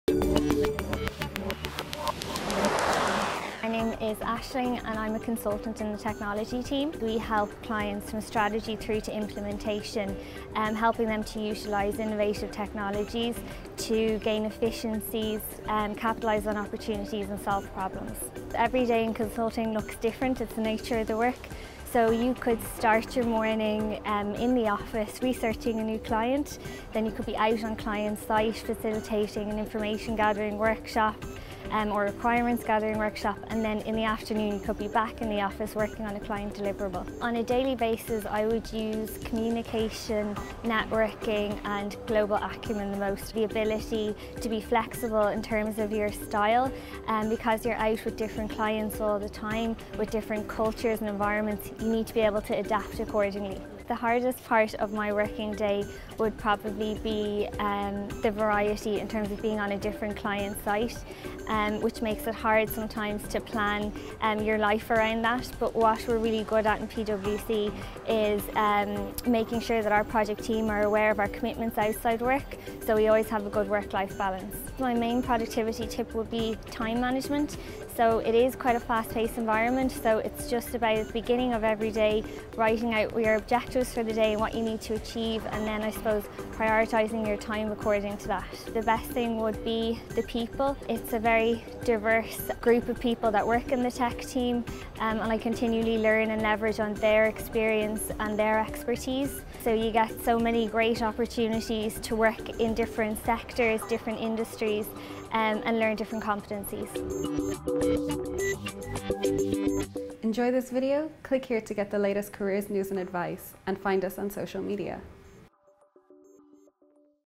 Aisling Tumelty, Technology Consulting, PwC
Aisling Tumelty is a technology consultant at PwC. She talked to Siliconrepublic.com about what it’s like to work at the company.